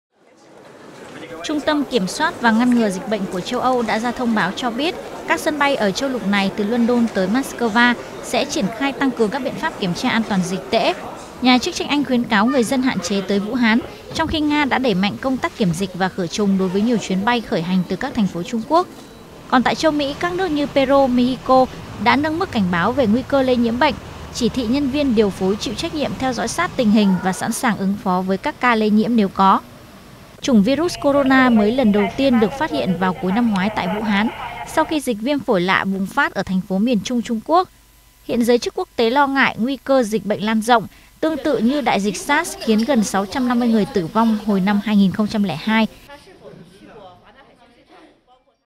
CÁC NƯỚC TĂNG CƯỜNG KIỂM TRA CHUYẾN BAY TỪ TRUNG QUỐC
Do dịch viêm phổi do virus corona diễn tiến phức tạp, hàng loạt quốc gia trên thế giới đã tăng cường kiểm tra đối với các chuyến bay đến từ thành phố Vũ Hán, Trung Quốc. Đây là các biện pháp đề phòng trước nguy cơ bùng phát thành đại dịch mới trên toàn cầu của loại virus gây chết người này.